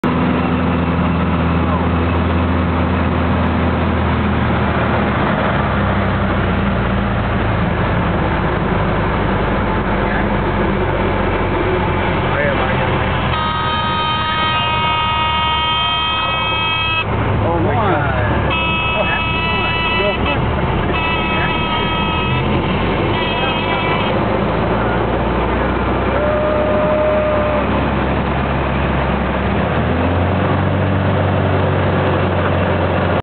Crazy Flight
This is what a power-on stall with a two full turn spin looks like.